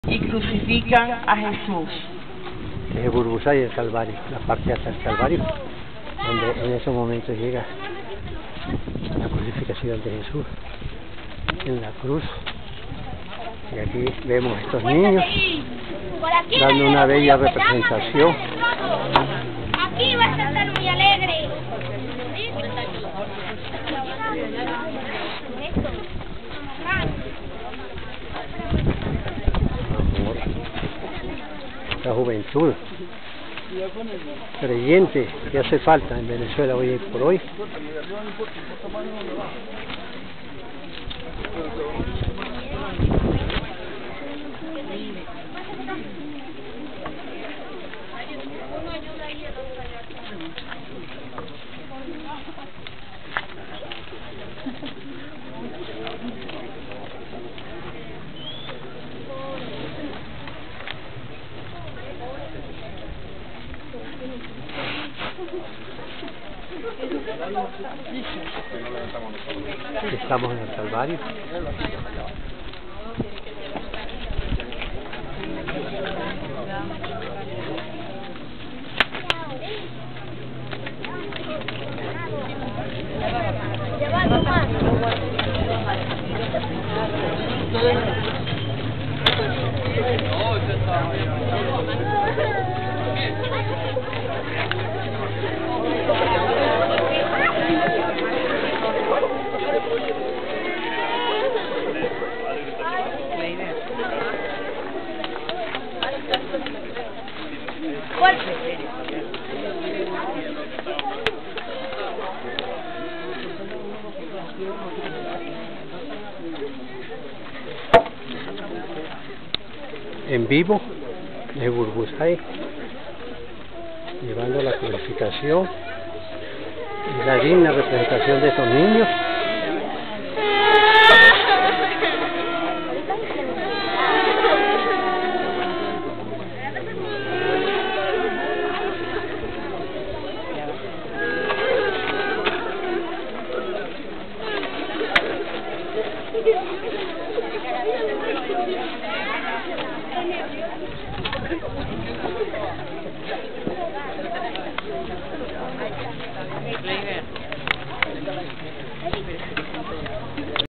PICT0027 SEMANA SANTA EN BURBUSAY,.
UN VÍA CRUCÉ,DE LOS NIÑOS QUE COMPENSAN EL SENTIRSE CERCA DEL SEÑOR,.QUE DIOS LES BENDIGA,.